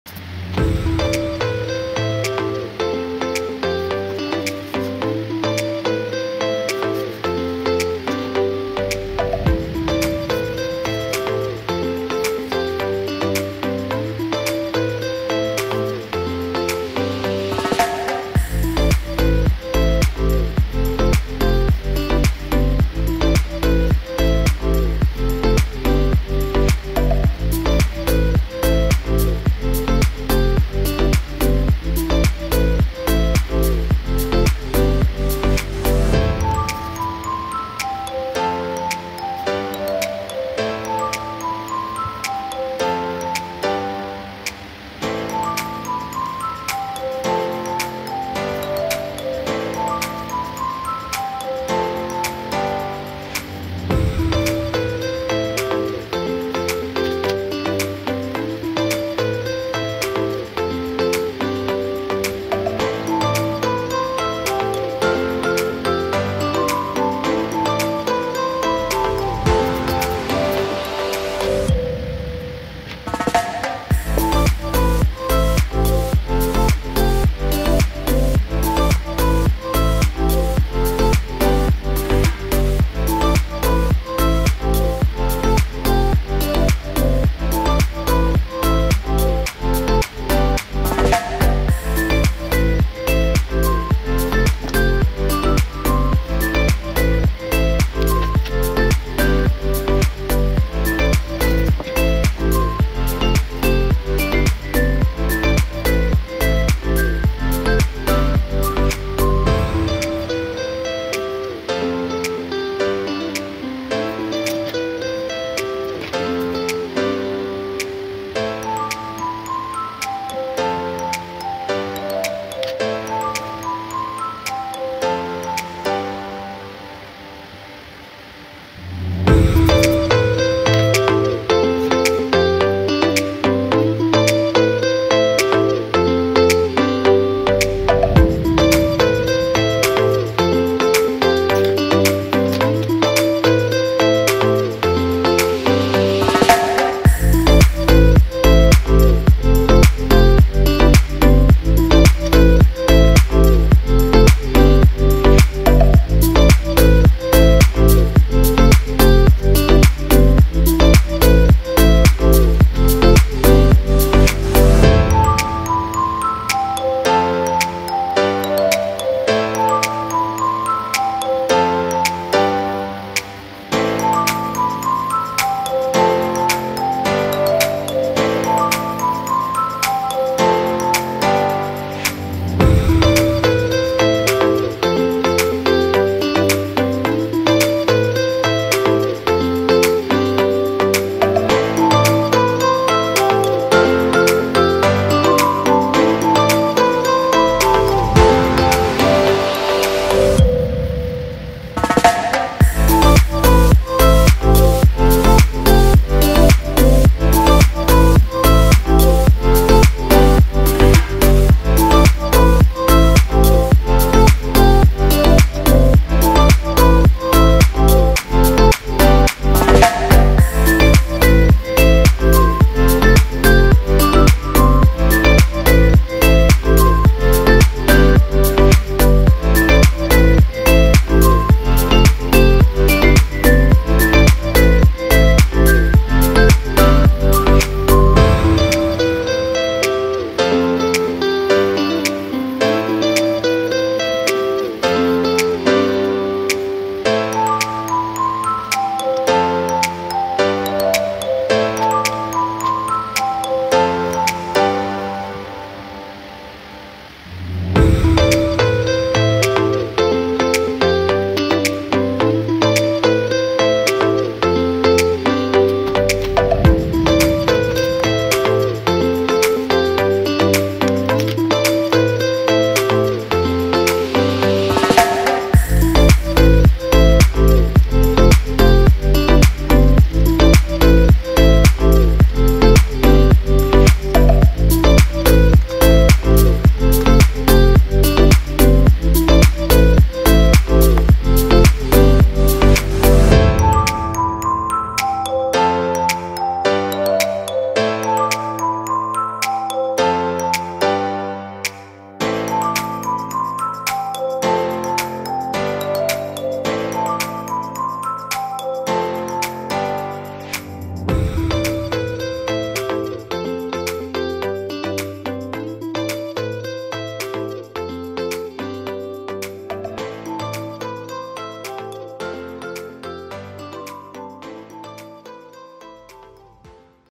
3D art: KJP ( kusal janith perera)🏏❤️
How to create a 3D design using paper and pencils.

 KJP, the short name of Sri Lankan cricketer Kusal Janith Perera, has been painted